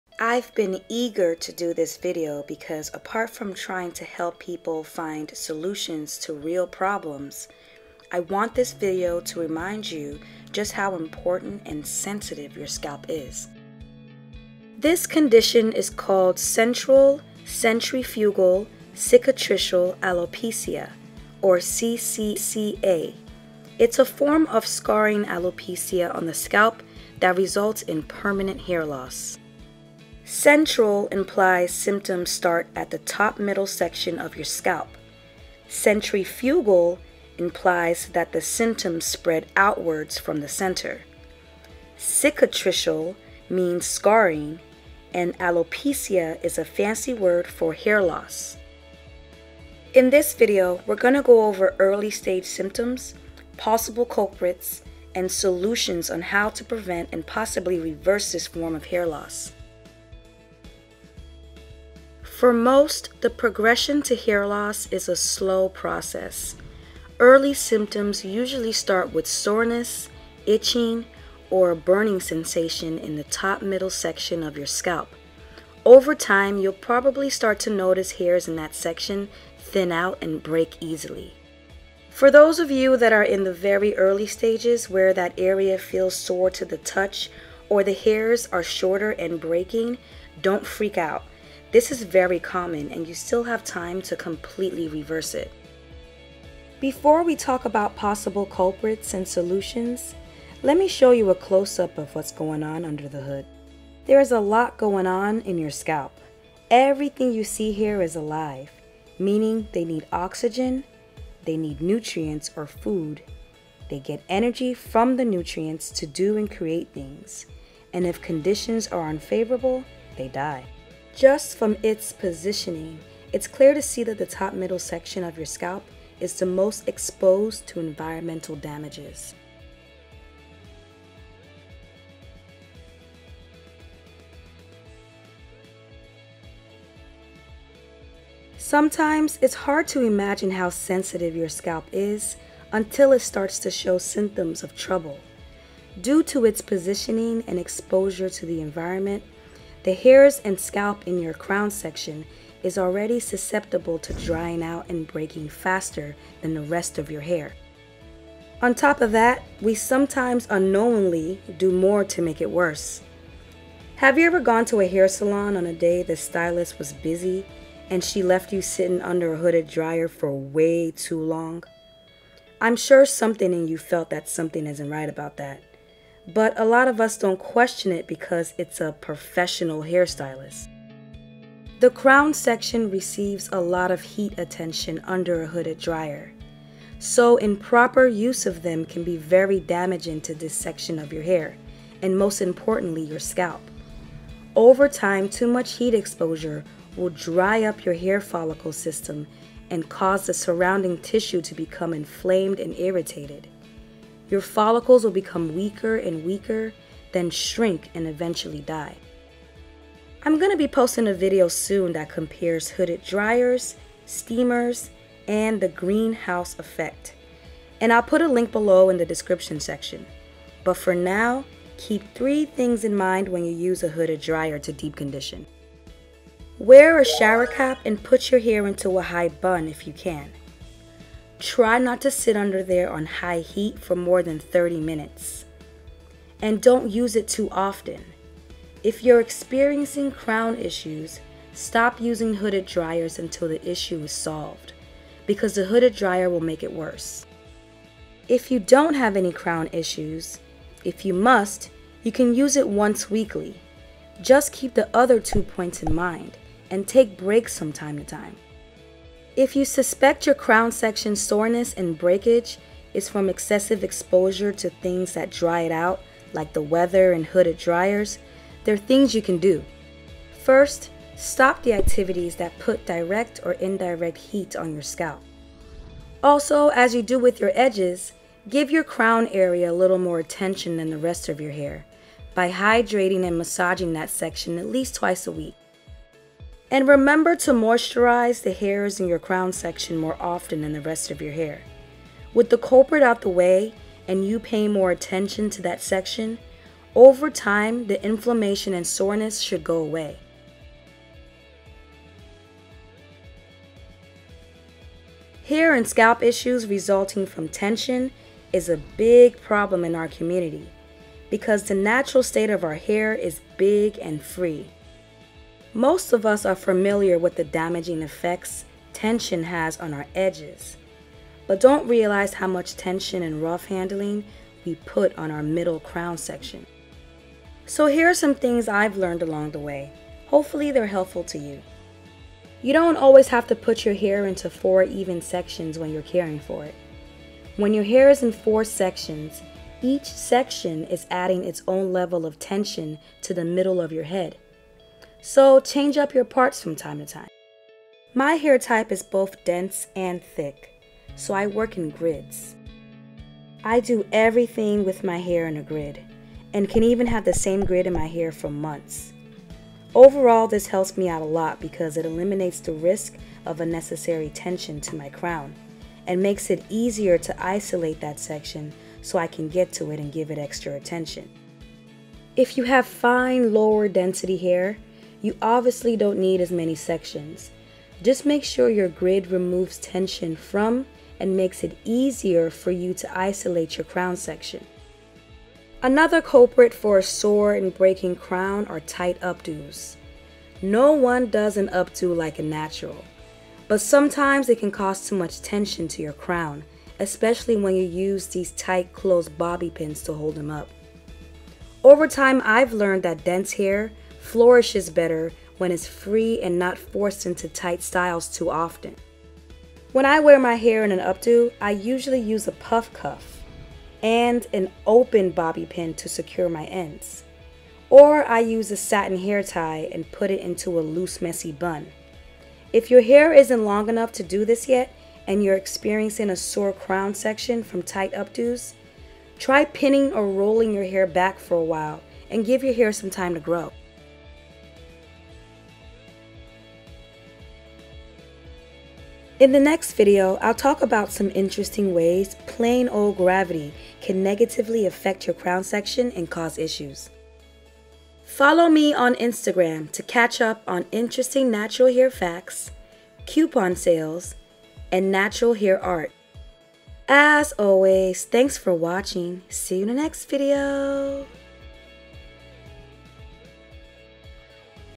Crown SORENESS and BREAKAGE - Causes and Solutions (Part 1)
Soreness, thinning and breakage at the crown area of your hair are due to a form of alopecia known as CCCA. We’re going to go over the early stages, possible culprits and how to treat it.

📗 REFERENCE 📗

Female Pattern Hair Loss and its Relationship to Permanent/Cicatricial Alopecia: A New Perspective
Exercise effects on cardiac size and left ventricular diastolic function: relationships to changes in fitness, fatness, blood pressure and insulin resistance
Effects of Regular Exercise on Blood Pressure and Left Ventricular Hypertrophy in African-American Men with Severe Hypertension